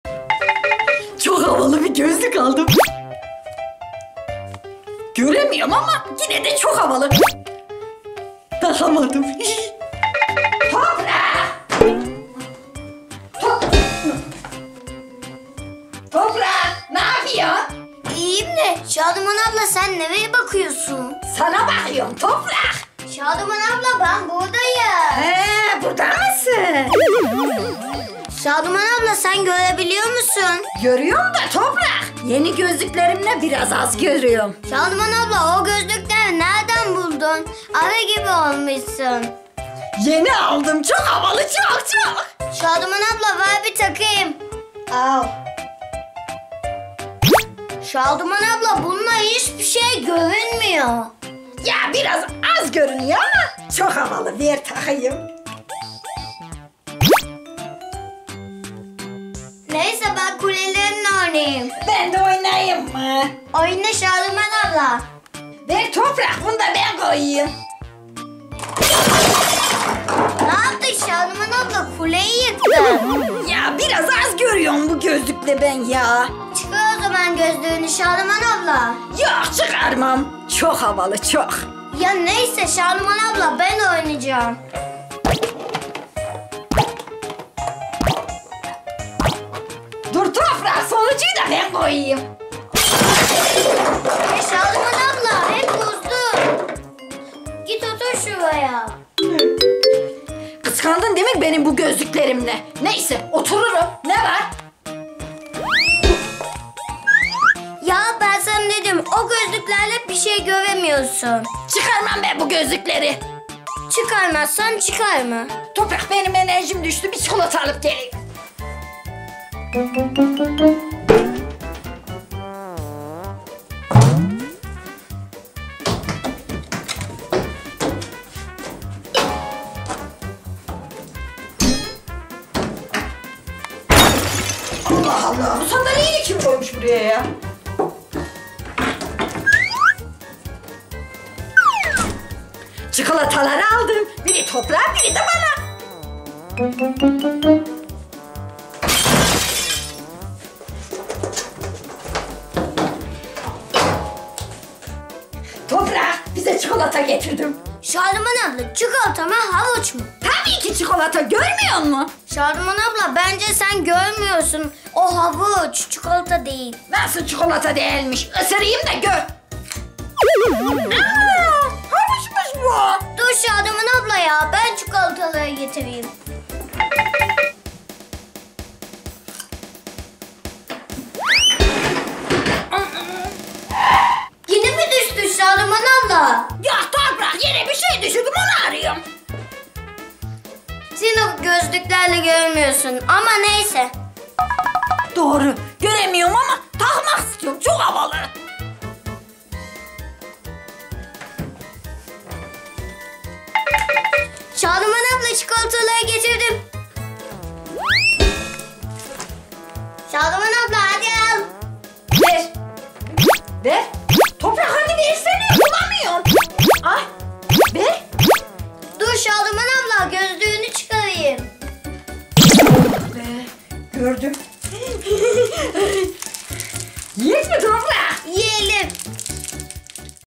Şaduman Abla Komik Gözlükleriyle Göremedi Toprak'ın Kulesini Yıktı.
Şaduman Abla havalı bir gözlük almış ama göremiyor her yere çarpıyor.Çikolata yerine havuç getiriyor Toprak'ta Şaduman Ablaya yardım ediyor.

Eğlenceli ve komik çocuk videoları için Toprak Yusuf'u takip edin